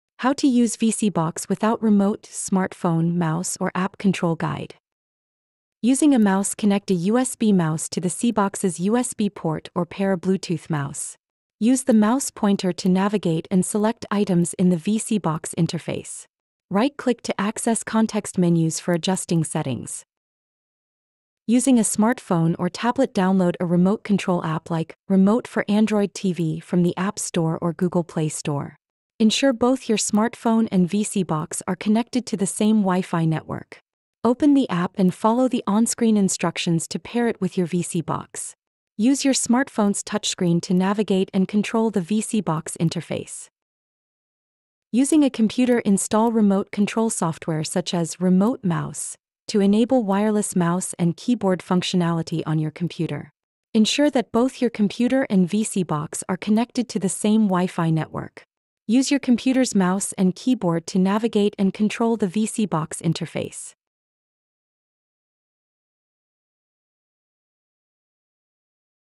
How To Use VSeeBox Without Remote – Smartphone, Mouse, Or App Control Guide
Discover how to effectively use VSeeBox without the need for a remote control in this comprehensive guide. In this video, we will walk you through various methods to operate VSeeBox using your smartphone, mouse, or app control. Whether you're looking to enhance your telehealth experience or streamline your virtual meetings, this tutorial provides step-by-step instructions to ensure you can navigate VSeeBox with ease. Join us as we explore the features and functionalities that make VSeeBox a versatile tool for remote communication.